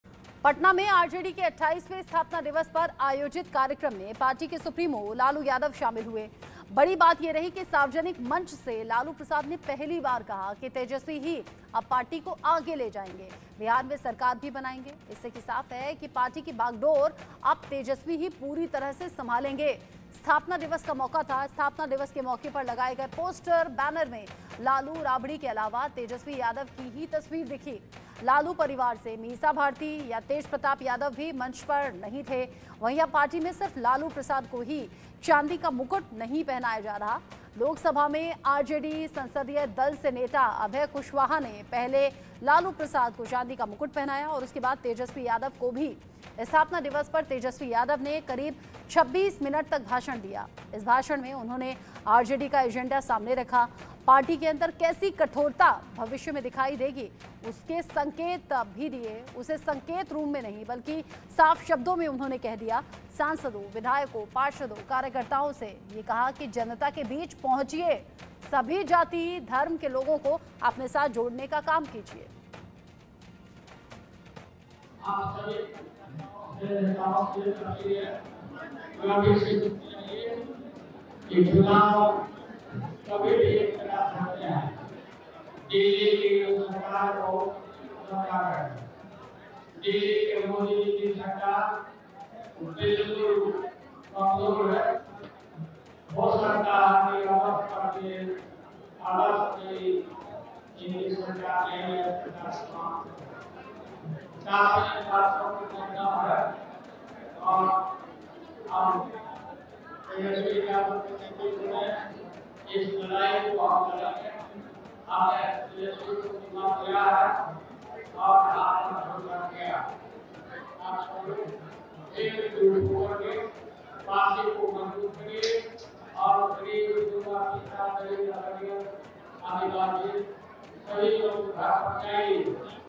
Lalu Yadav ने Tejashwi के हाथों में सौंपी RJD की बागडोर | Bihar Politics | India News
RJD ने आज अपना 28वां स्थापना दिवस मनाया. इस मौके पर पटना में पार्टी ऑफिस में लालू और तेजस्वी यादव को चांदी का मुकुट पहनाया गया. इसके साथ ही लालू प्रसाद यादव ने अब RJD की बागडोर तेजस्वी यादव के हाथों में सौंप दी है. लालू प्रसाद ने पहली बार कहा कि तेजस्वी ही अब इस पार्टी को आगे ले जाएंगे और बिहार में सरकार बनाएंगे.

RJD celebrated its 28th foundation day today. On this occasion, Lalu and Tejashwi Yadav were crowned with silver crowns at the party office in Patna. With this, Lalu Prasad Yadav has now handed over the reins of RJD to Tejashwi Yadav. Lalu Prasad said for the first time that Tejashwi will now take this party forward and form the government in Bihar.